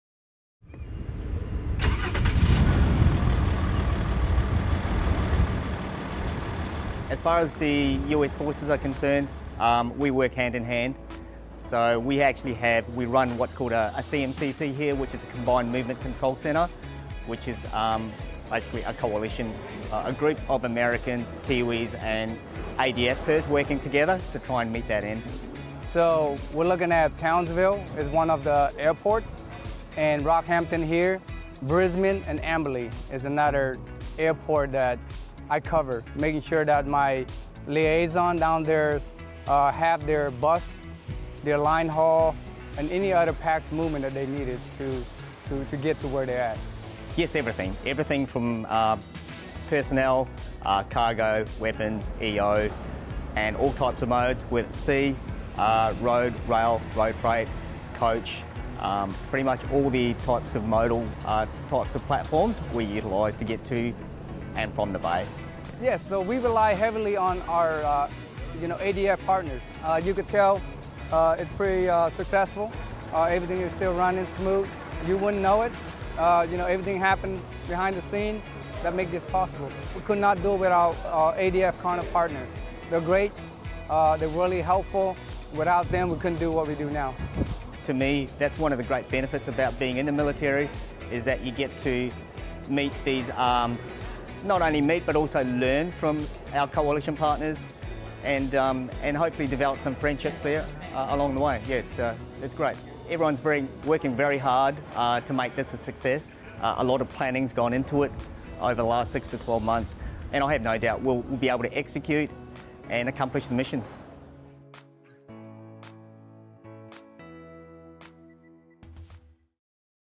TS19 Combined Movement Control Center (2019) 🇺🇸
🔴 [Author]:
Video by Staff Sgt. Emili Koonce

🔴 [Location]:
Australia

🔴 [Description]:
Australian Defense Force, U.S., New Zealand as well as other coalition partners worked together for over 6 months planning Exercise Talisman Sabre 19. Now on the ground during the execution phase U.S. Army Master Sgt. Vantha Chhim and his counterparts to include Australian Army Cpl. Gerardo Clarkson are continuing to make sure equipment and personnel are where we need to be.

🔴 [VIRIN]:
190712-F-UH796-001